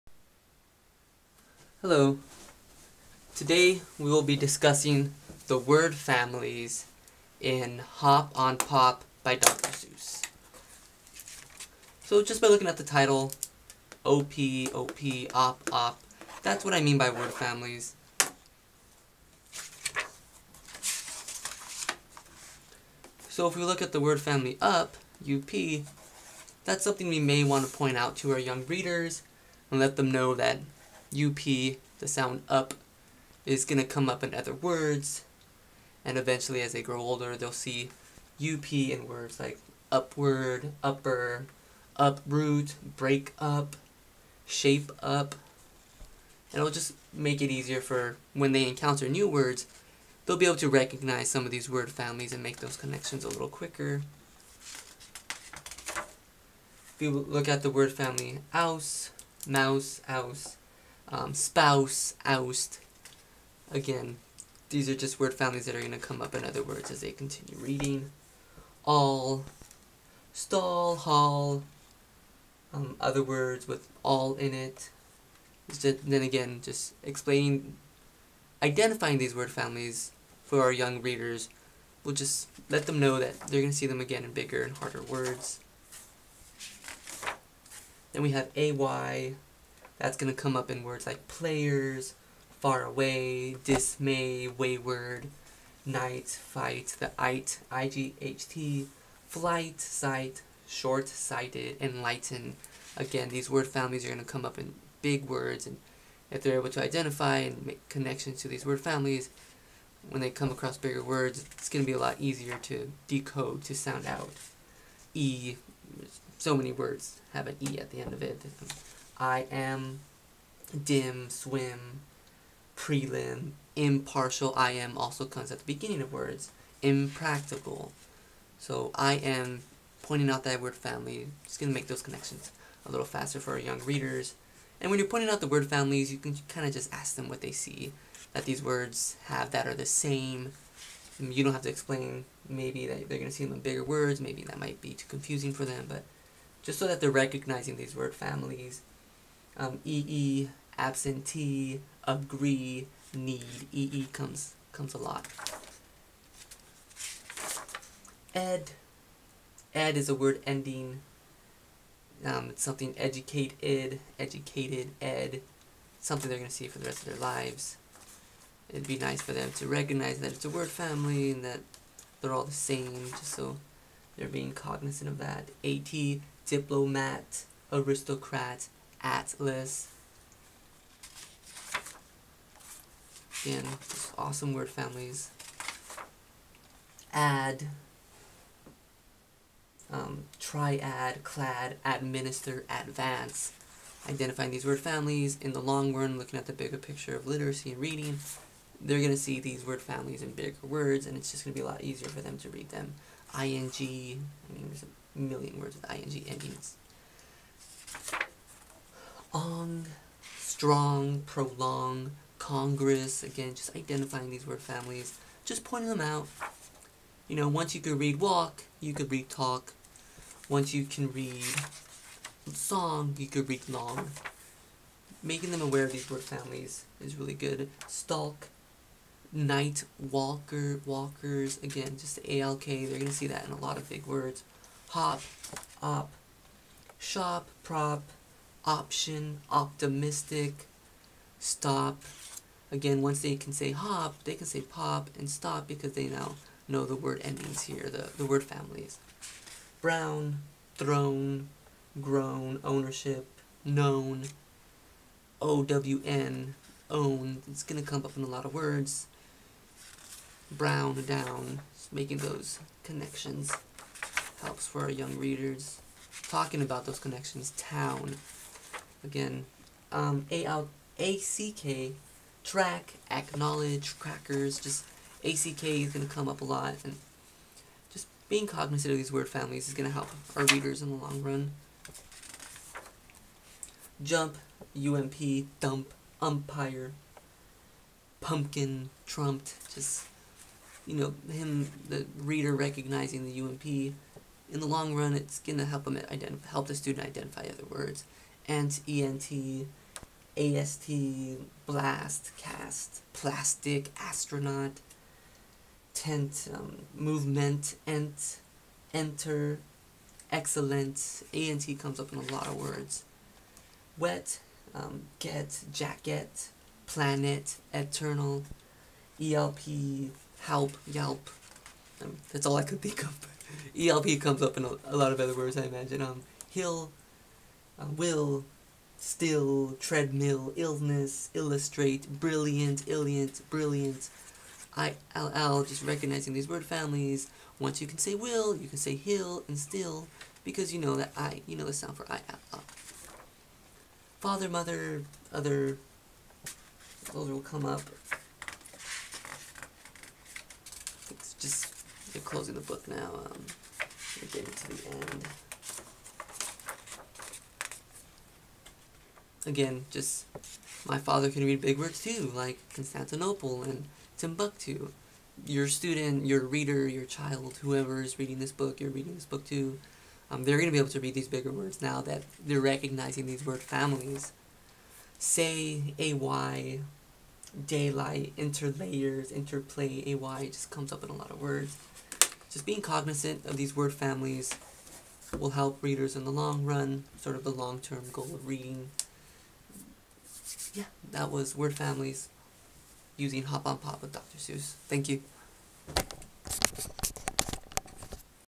Hop on Pop by Dr. Seuss- Word Families
We will be discussing word families in Hop on Pop. An example of a word family: -op found in hop and pop. The "op" will be found in other words as your reader progresses with reading. In words like "OPtion, OPtimistic, shOP, prOP, etc.
Having your reader be aware of word families will help them decode, sound out words more automatically and allow for the reader to focus on comprehension. Thanks!